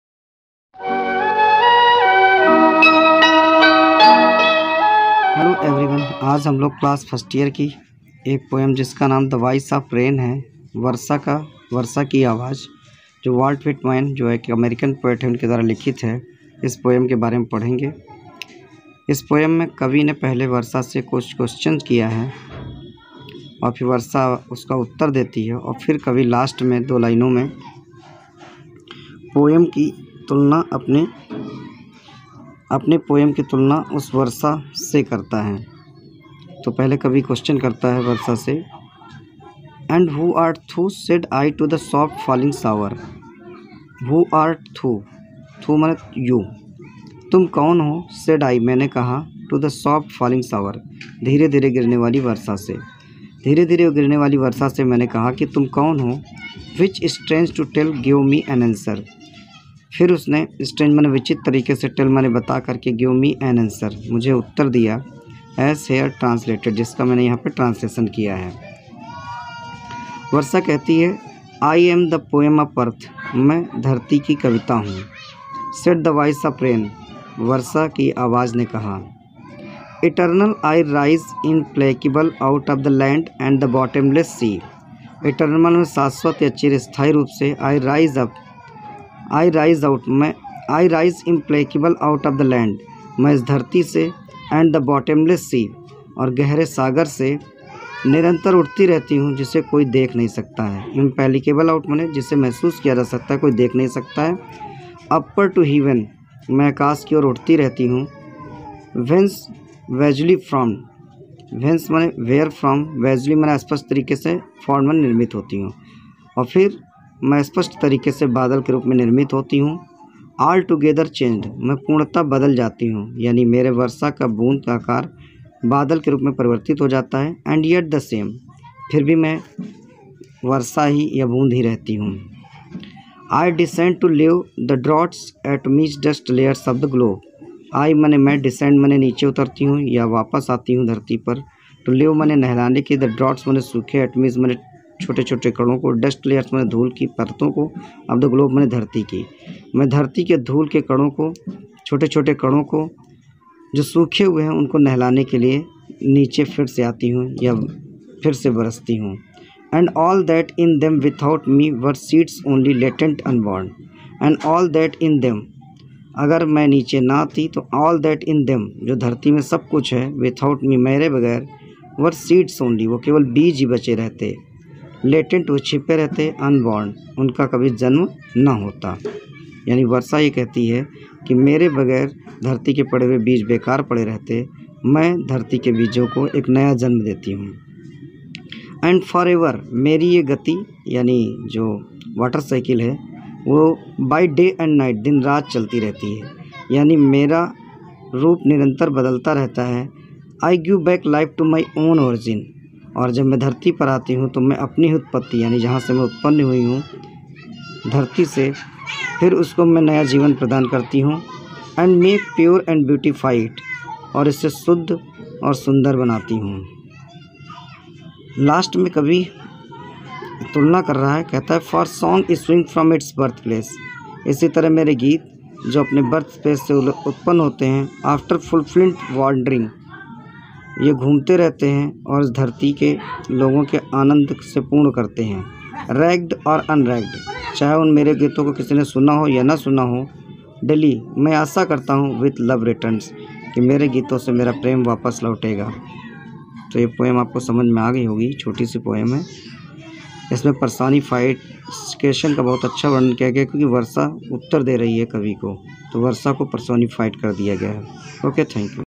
The voice of rain by Walt whitman
Is poem ke antargat ham log Walt whitman ki poem The Voice of rain ke bare mein padenge jo class first year mein UP Board mein chal rahi hai